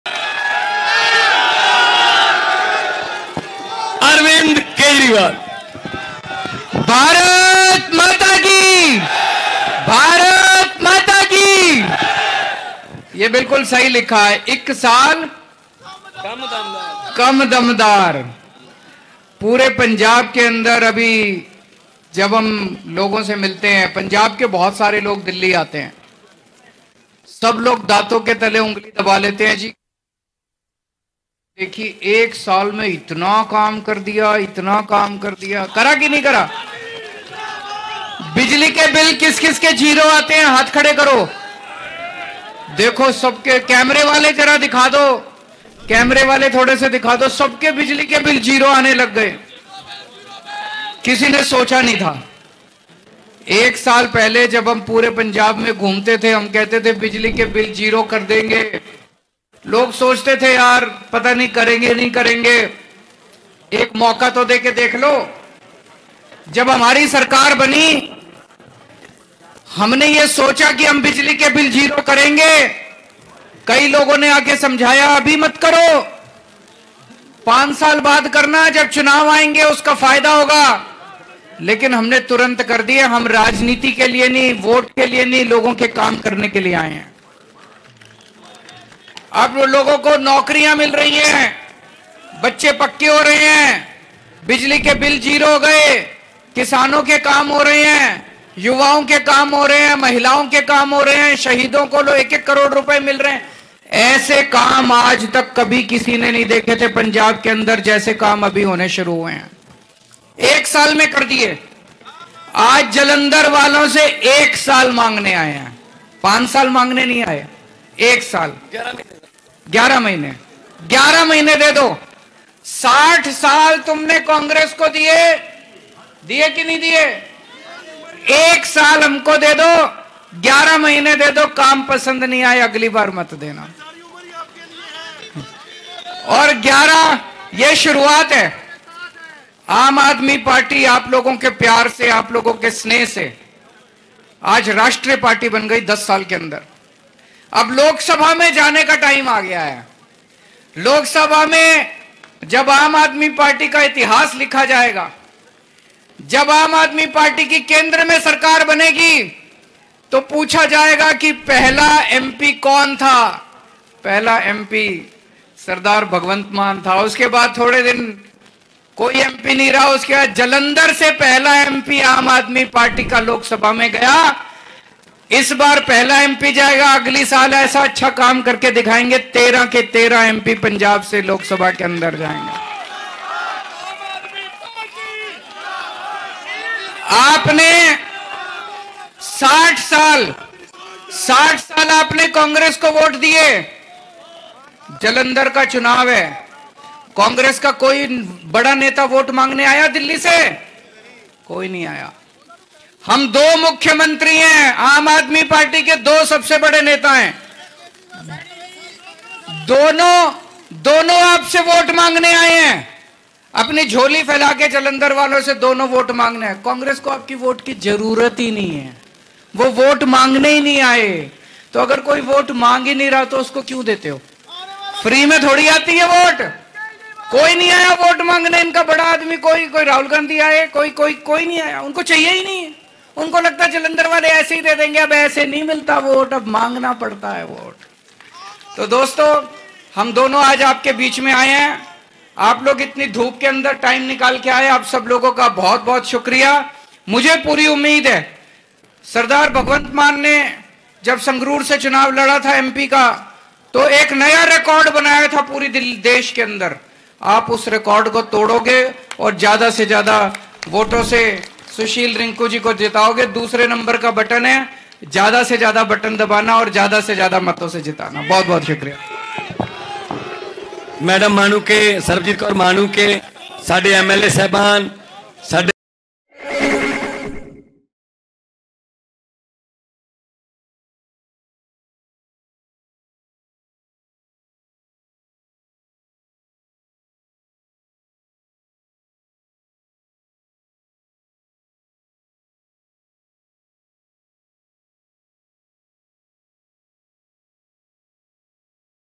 Jalandhar Central में Arvind Kejriwal और CM Bhagwant Mann का दमदार Roadshow I AAP Newsify Live
More :-

Video Credit : - AAP YT Page
Editor : - Vishal Singh
Producer :- Newsify